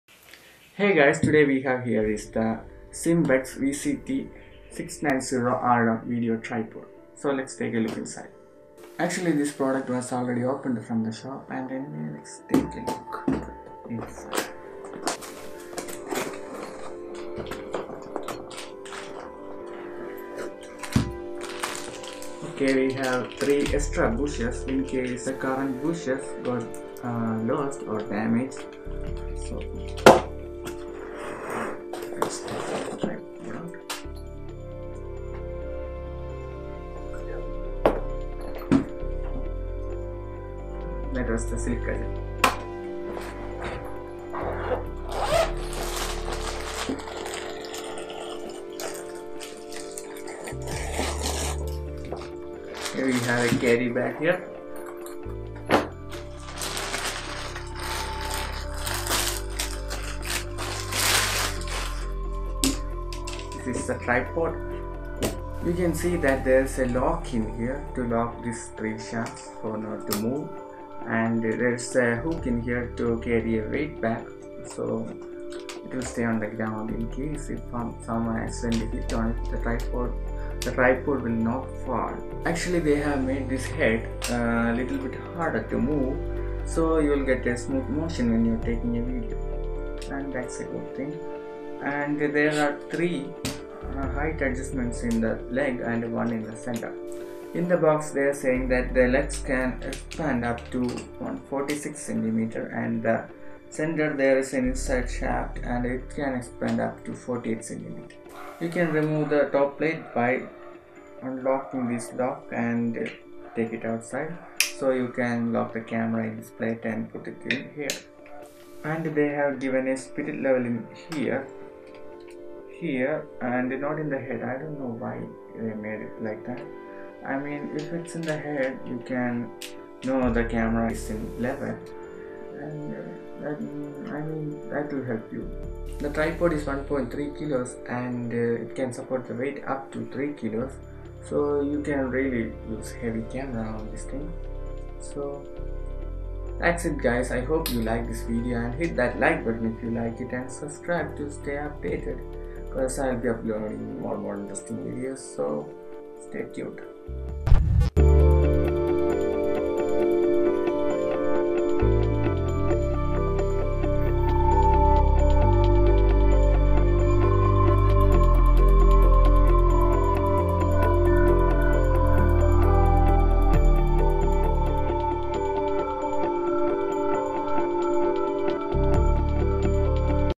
Simpex VCT 690RM Video Tripod Unboxing and Review | Best Budget Tripod
In this video i am unboxing and reviewing Simpex VCT 690RM Video Tripod.
This is the best Tripod for the budget.
INR-2,500
$-34